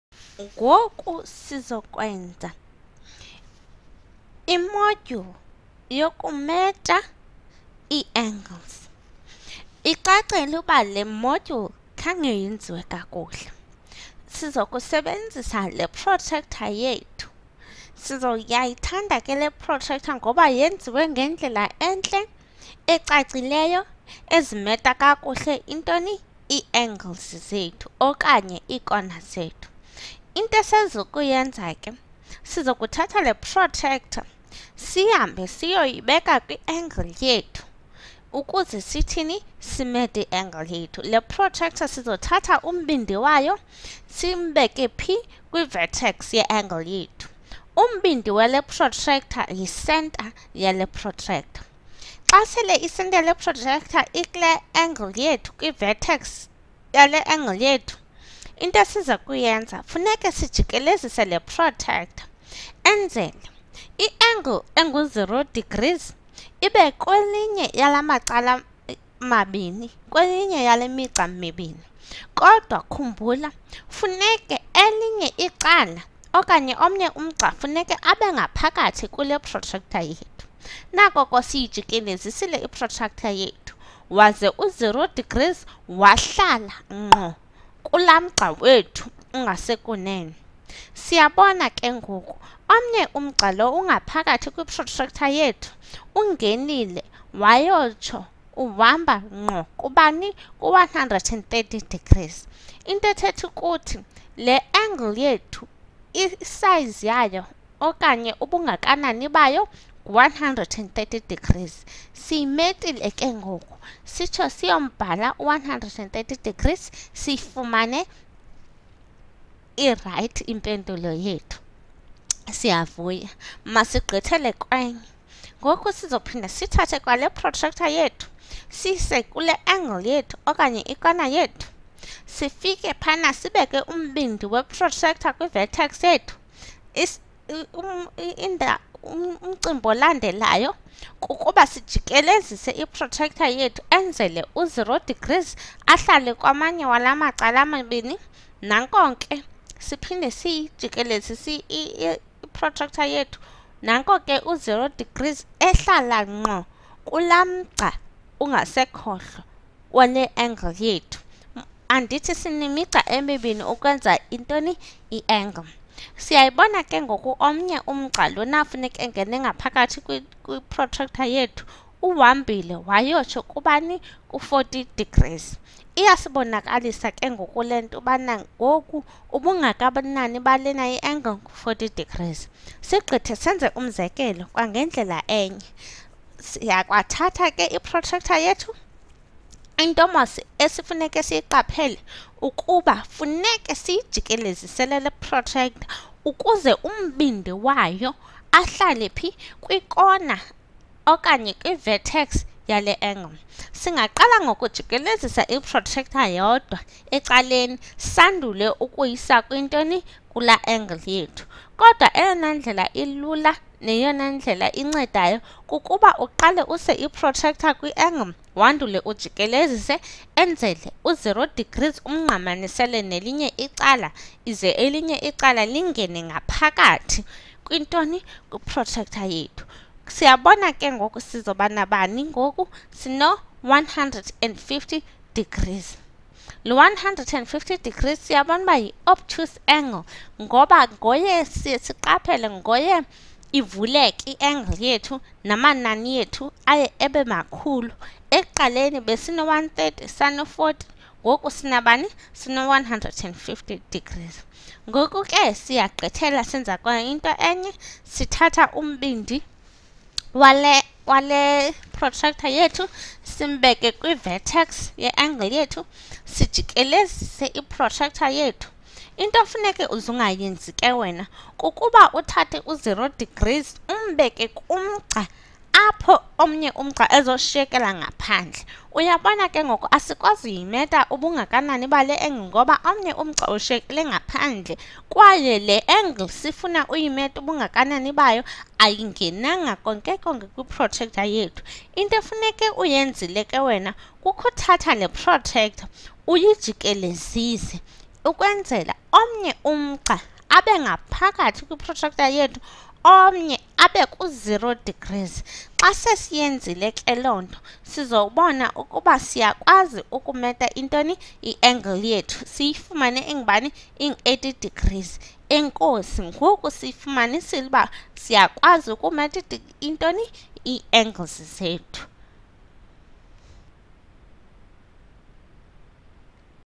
Measuring angles
This original Khan Academy video was translated into isiXhosa by Nolusindiso Mpoyiya. The translation project was made possible by ClickMaths